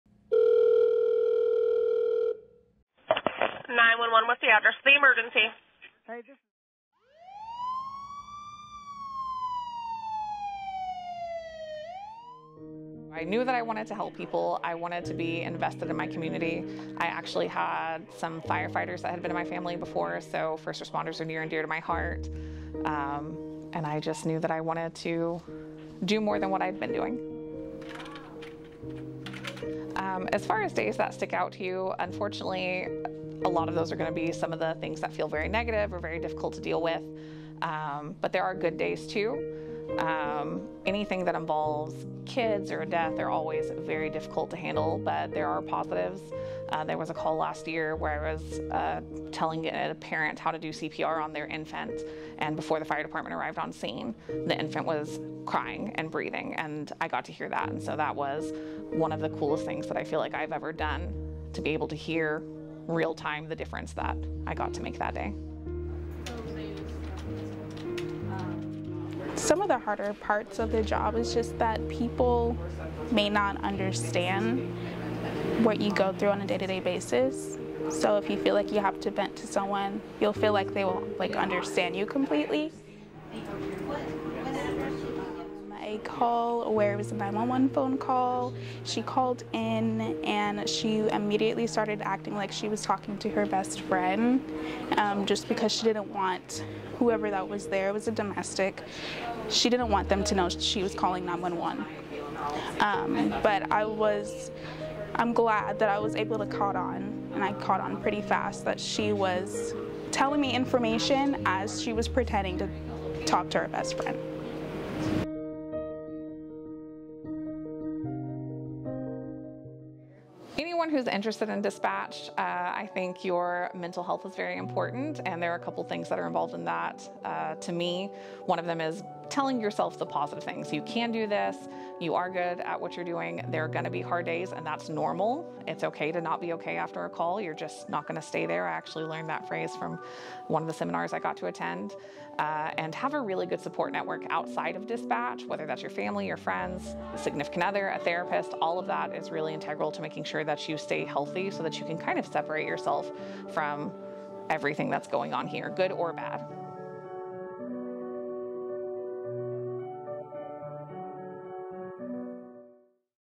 Thank you to our dispatchers...
As National Public Safety Telecommunicators Week wraps up, we're taking a moment to express our heartfelt appreciation for the heroes behind the scenes: our dispatchers.

They're the calm voices in the chaos, the lifelines in emergencies, and the unseen support ensuring that help reaches those in need swiftly and efficiently. From managing distress calls to coordinating responses, they play a vital role in keeping our communities safe.

So, here's to our dedicated dispatchers – thank you for your unwavering commitment and invaluable service.

With limited exception, all use of City of Lawton content (text, photographs, graphics, video, etc.) requires the permission of the City of Lawton.